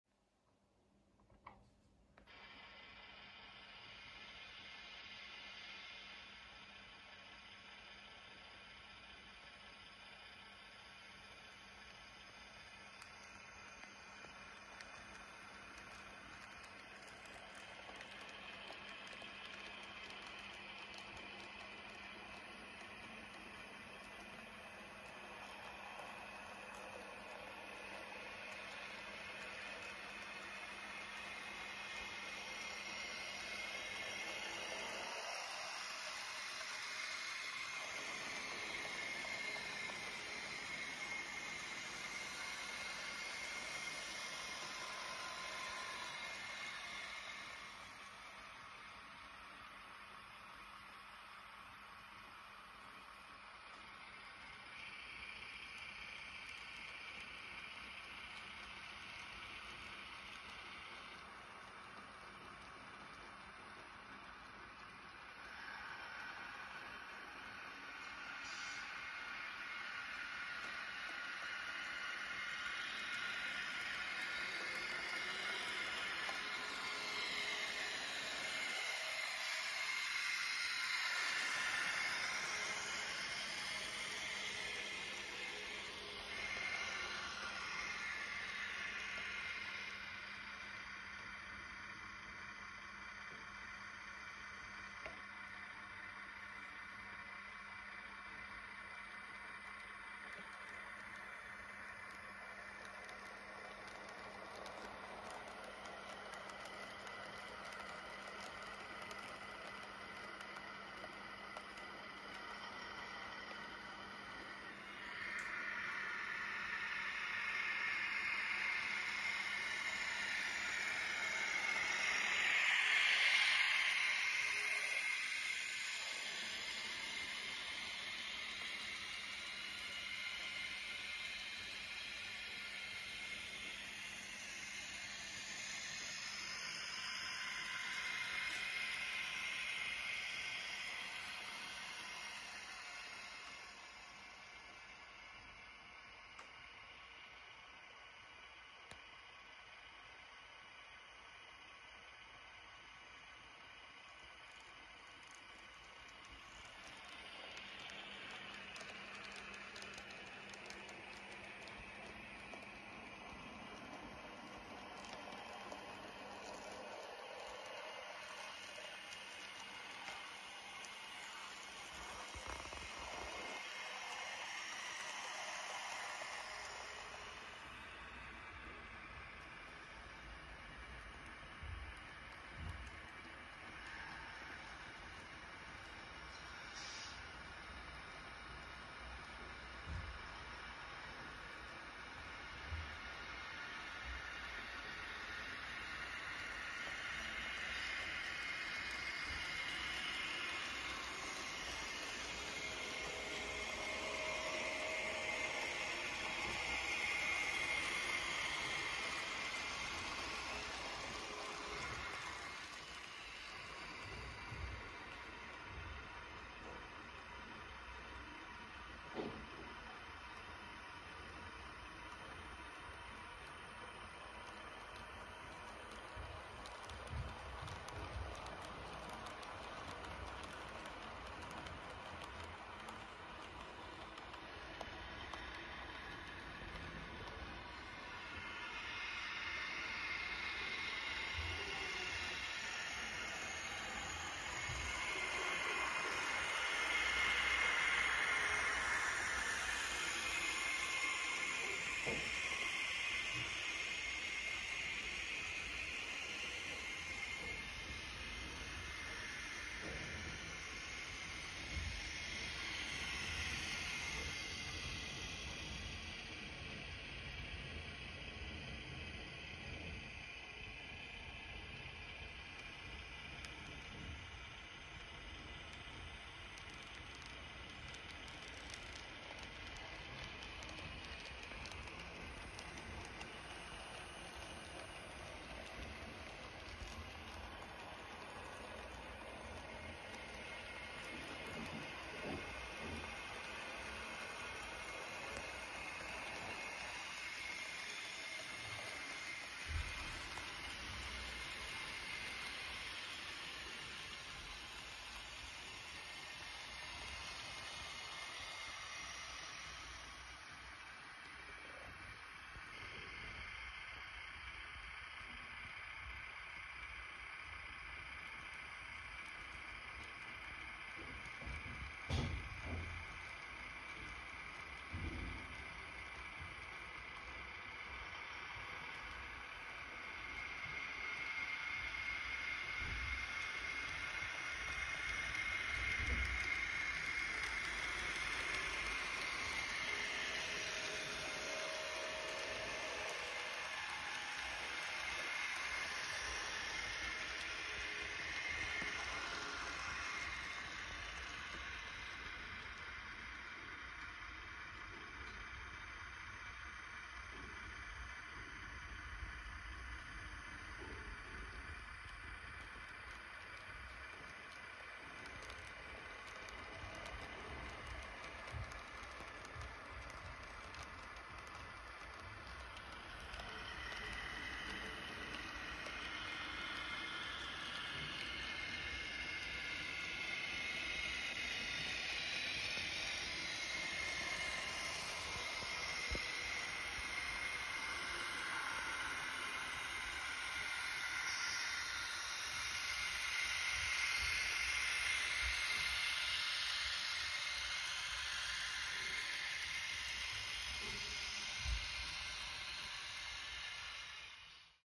Z briefcase #032 - Night, Early Morning , Daylight (Märklin Z)
Z briefcase #32 - Night, Early Morning , Daylight (Märklin Z).
Testing low lighting to simulate Early Morning trains
you can easily see the light beams end enlighted cabin